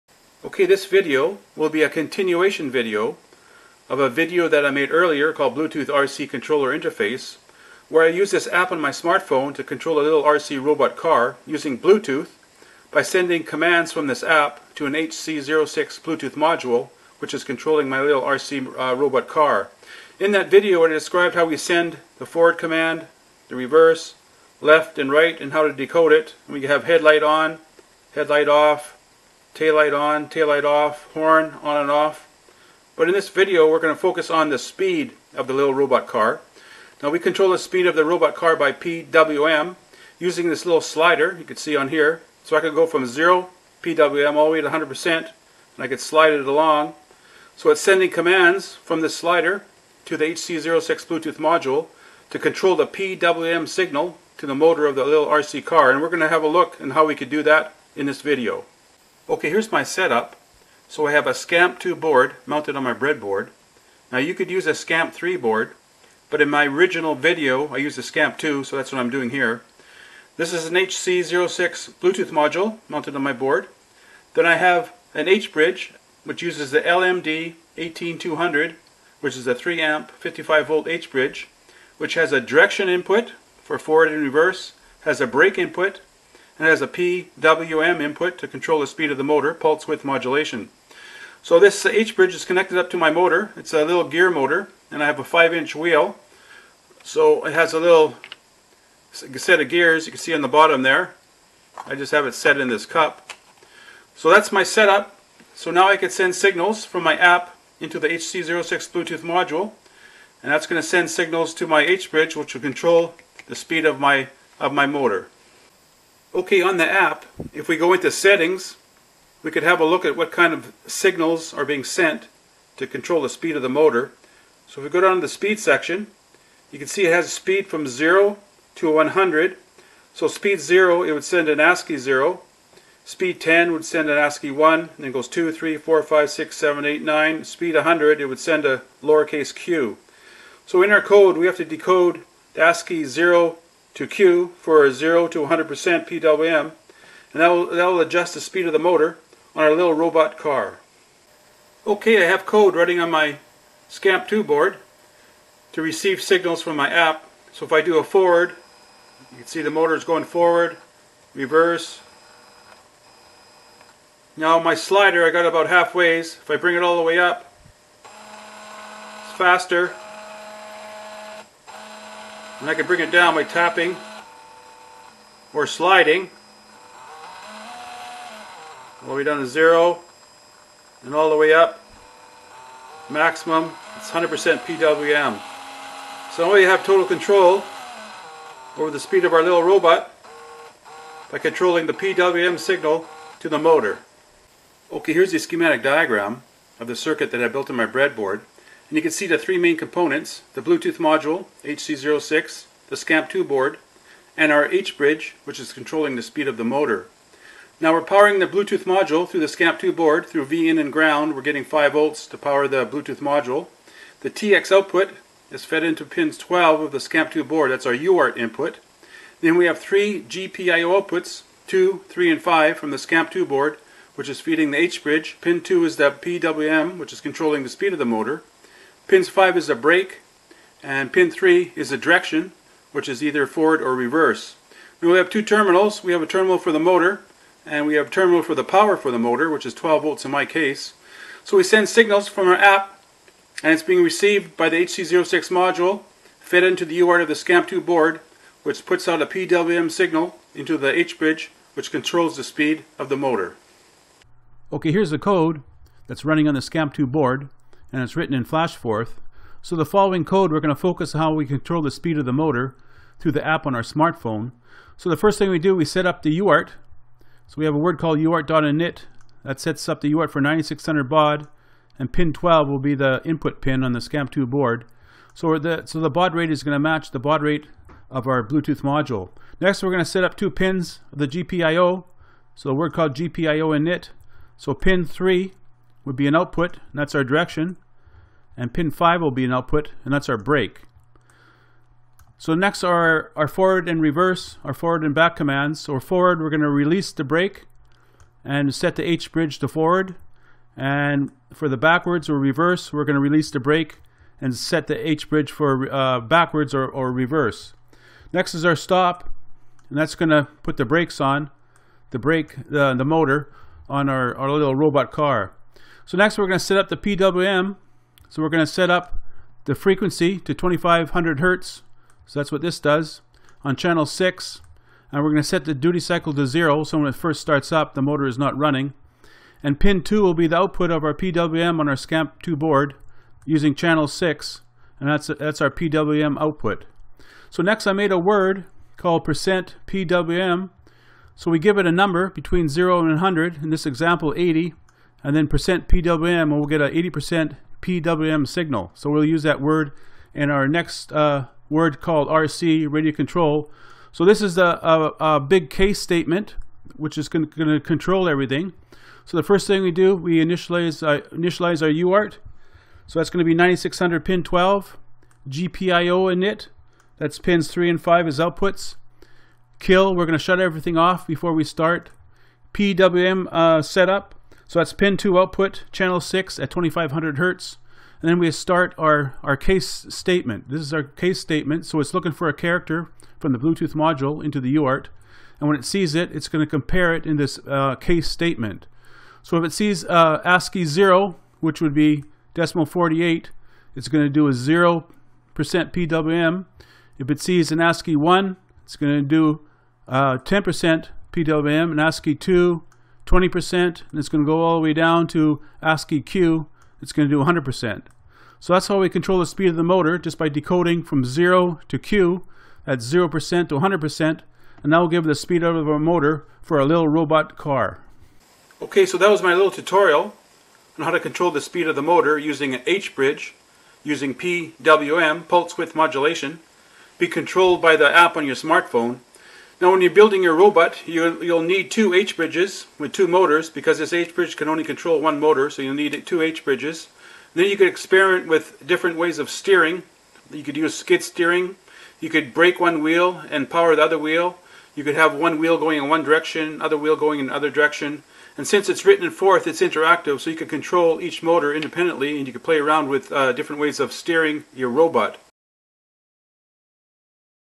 DC Motor Speed Control / H-Bridge / PWM / Scamp FlashForth
This video will describe how to control the speed of a DC motor using an H-Bridge and PWM. A smartphone app using Bluetooth will be used to send speed control data wirelessly for RC robotic applications. FlashForth running on a Scamp board decodes the speed data and generates the appropriate PWM signal.